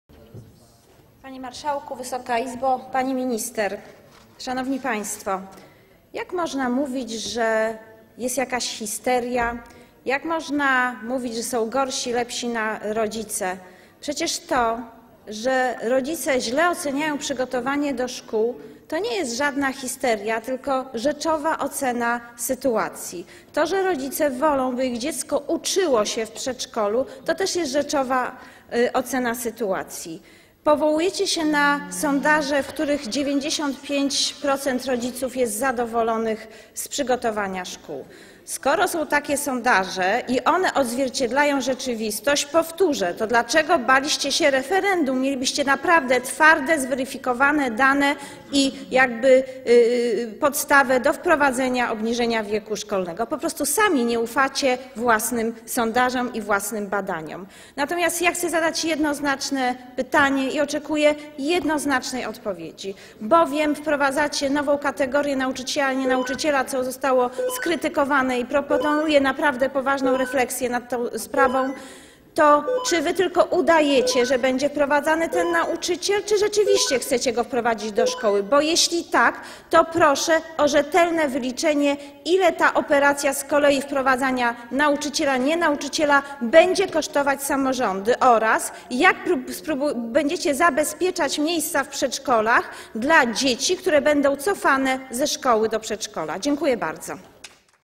Marzena Machałek - wystąpienie z 19 marca 2014 r.
Popierasz? Udostępnij! Marzena Machałek - wystąpienie z 19 marca 2014 r.

Sprawozdanie Komisji o poselskim projekcie ustawy o zmianie ustawy o systemie oświaty